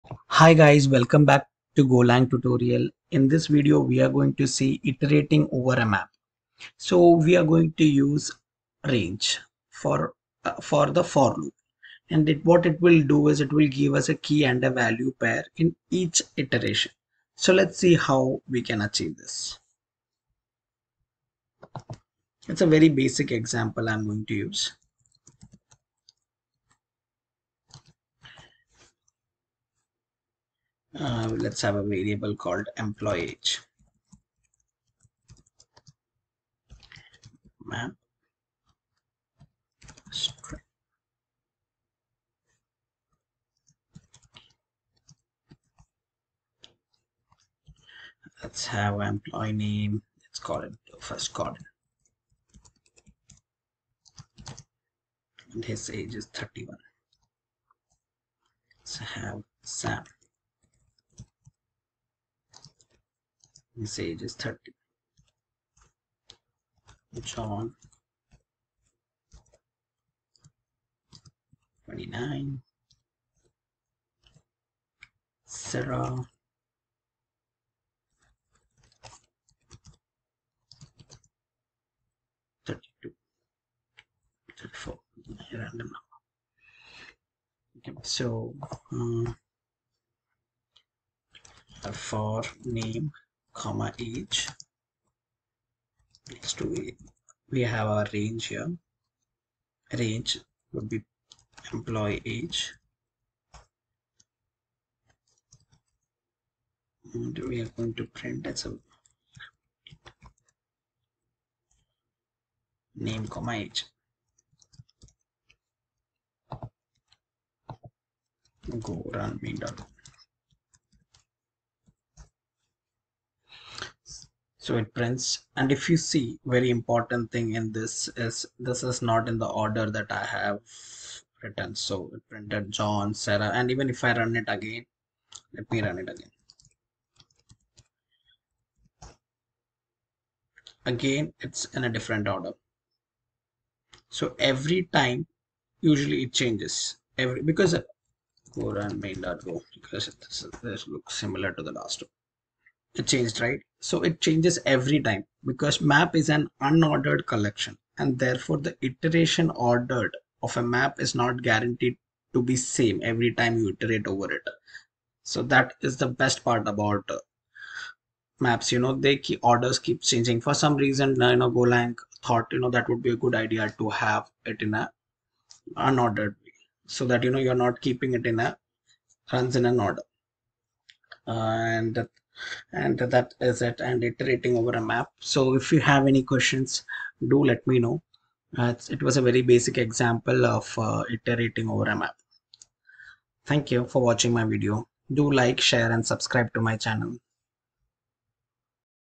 Golang maps | Iterating over a Map | Golang Tutorial for Beginners
Iterating Over a Map.
You can iterate over a map using "range" form of the for loop. Its gives the key , value pair in every Iteration.
Map is an unordered collection and therefore iteration order of a map is not guaranteed over to be same every time we iterate.
Let me know if you have any questions in Maps.